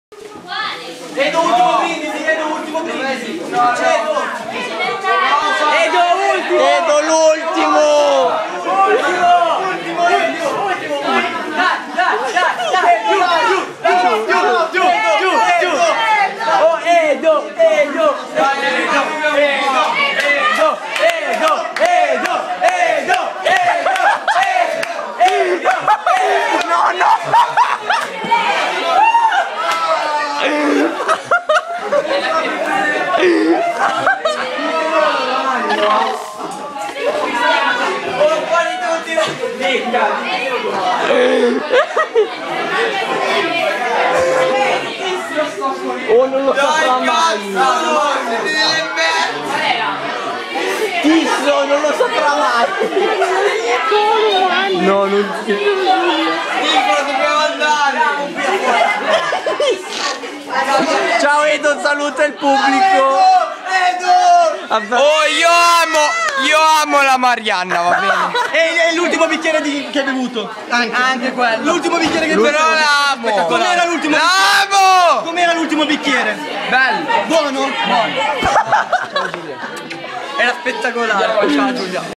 Qualcuno ha bevuto?
Un edo a caso ubriaco dopo tanti bicchierini ahahah